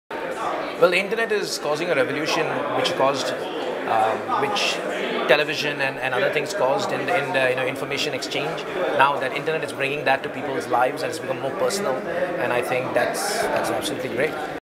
Internet Future in a Nutshell? IETF 2015: Ritesh Mukherjee
Ritesh Mukherjee, Director of Network Solutions at Huawei Technologies Canada, shares his answer to the question “Using just a few words, in just five seconds or less, describe the future of the Internet.” at Internet Engineering Task Force meeting 92, which took place in March 2015 in Dallas, Texas. Recorded by the Imagining the Internet Center, an initiative of Elon University, North Carolina, USA.